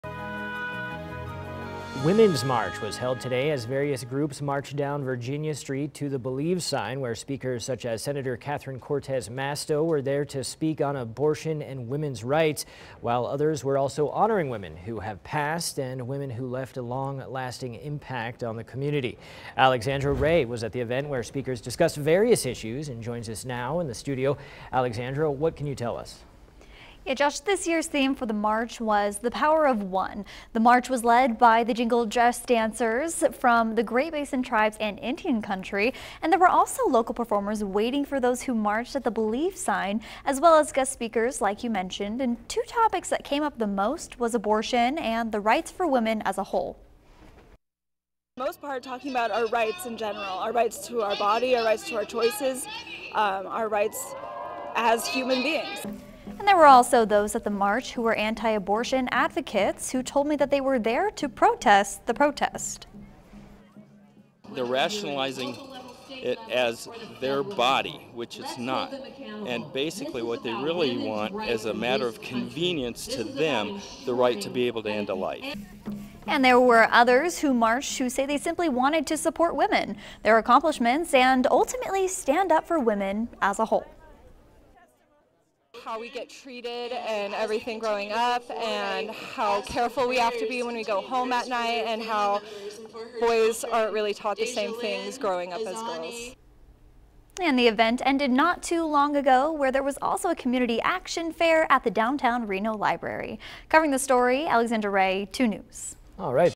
7th Annual Reno Women’s March
The march was led by the Jingle Dress Dancers from the Great Basin Tribes and Indian Country.